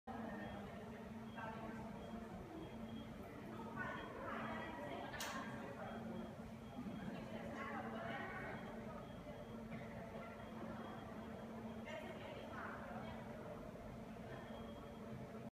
The Magic Planet
Den Magiske Planet giver dig mulighed for at komme tæt på de forskellige planeter i vores solsystem. Kom og prøv på Steno Museet, Aarhus